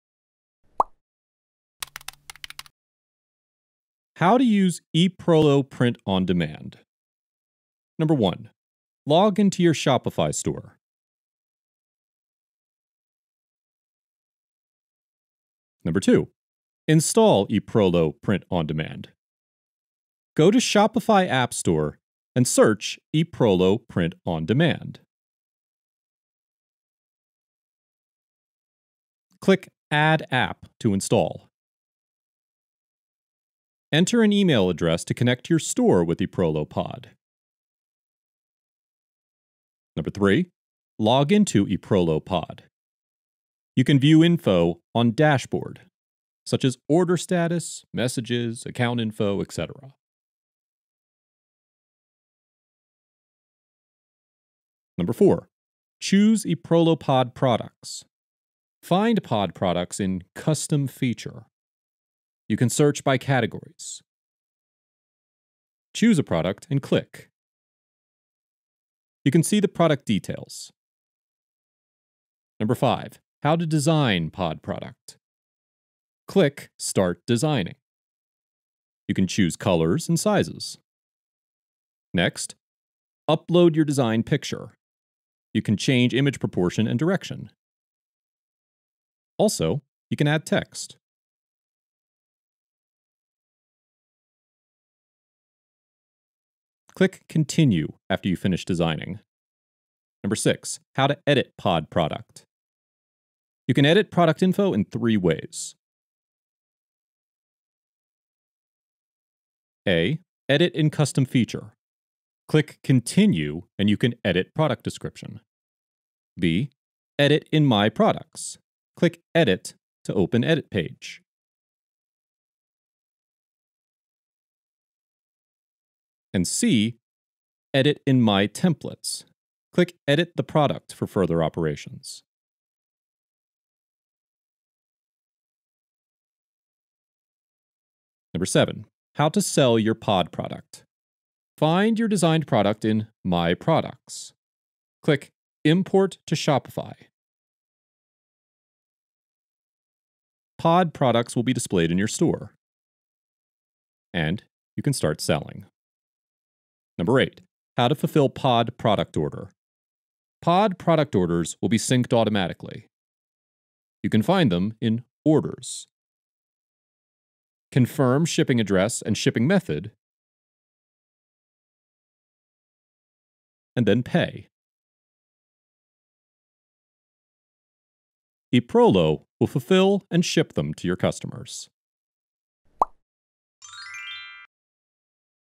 How to Use EPROLO - Print on Demand on Shopify
EPROLO has created a free app of Print on Demand for Shopify stores. Print on Demand dropshipping is hot due to the increasing online business. If you’re one of those who are interested in it, this is what you want. Here, you can learn how to find and design products for your stores.

▏What is Print on Demand?
Print on Demand (POD) is a business process that allows dropshipping store owners to add self-designed logos or patterns on products, such as T-shirts, bags and accessories. Selling print-on-demand products bring benefits to the stores: distinctive features, an impressive image, and branding.

▏EPROLO POD provides the following services:
● Find Print on Demand products
● Factories in America, Australia and China
● Fast delivery in 5-10 days
● Design and edit your own products
● Auto fulfill orders
● Track packages
● No MOQ (Minimum Order Quantity)
● Quality Control

▏How to use EPROLO POD (step-by-step):
1. Log into your Shopify store and Install EPROLO ‑ Print On Demand
You can search the app in Shopify App Store, or visit EPROLO official website to register.
2. Log into EPROLO POD and choose products
It will connect your store with the app. After login, you can find products in Custom Feature. It’s on the left of the dashboard.
3. Design your product and edit the description
Choose a product you want, such as clothing, bag, and etc. Click it and enter the design page. You can upload your designed picture and place it on the product. You can also add texts. Click Continue after finishing the design, and you will move to the editing page.
There are three ways to edit a product description. You can check it in the video.
4. Push your products into Shopify store and sell
You can push your designed product with a few clicks. Find the product in My Products and click Import to Shopify.
5. EPROLO will take over order fulfillment
When you get orders on your store, EPROLO POD will sync them in Orders. You need to confirm the shipping address and choose a shipping method. After you pay, the team will produce, package, and ship the product to your customer.